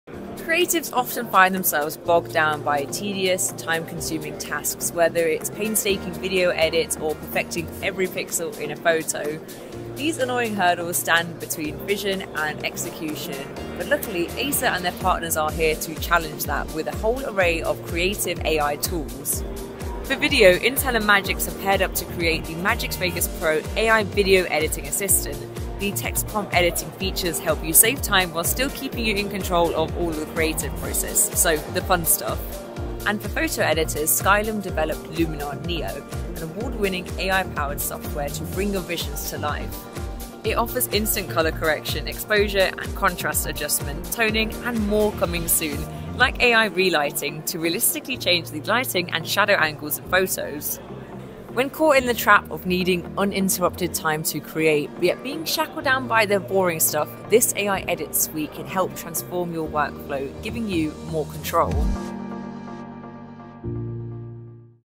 Next@Acer 2024 | Get Creative with Skylim on Acer
Never settle for less than your vision! 🔥 Skylim’s AI tools on our new Acer AI PCs make every edit a breeze—smooth, smart, and seamless. No more wasted hours on boring stuff; now, you can focus on bringing every detail to life. 🎨✨
And guess what? Talia’s here to show you all the magic.